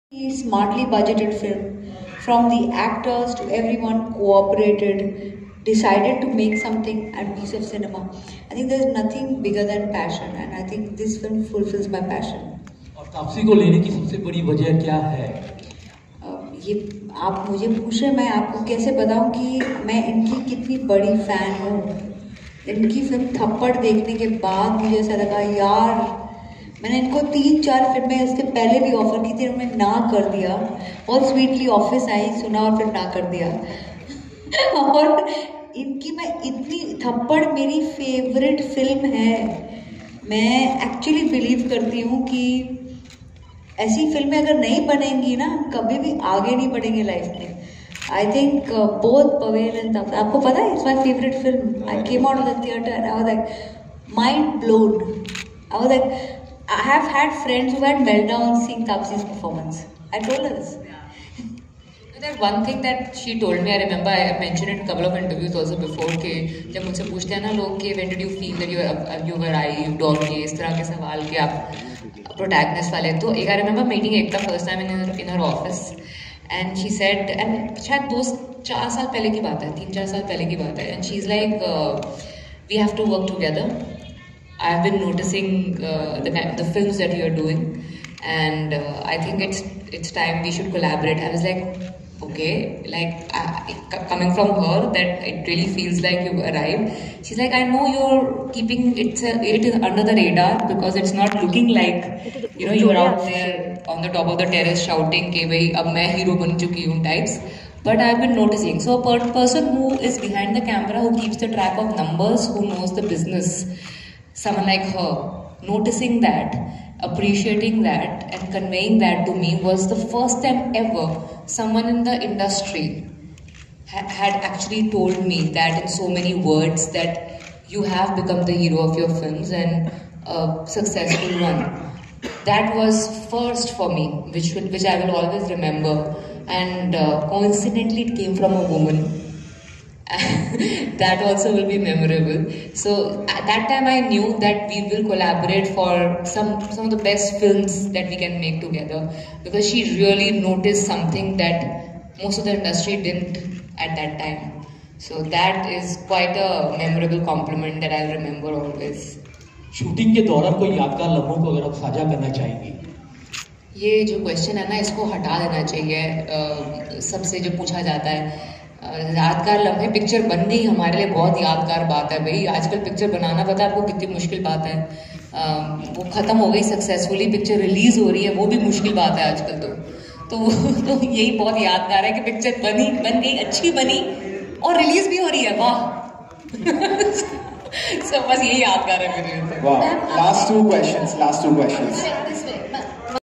Film "DOBAARA" promotion in Kolkata, The City of Joy...!!!
The team of Hindi film "Dobaara" was in Kolkata for the promotion activities. The film is already released and running in theatres successfully...!!!

Video by; Sukanya Banerjee
Perfect News, Always Sukanya
DOP; 29/8/2022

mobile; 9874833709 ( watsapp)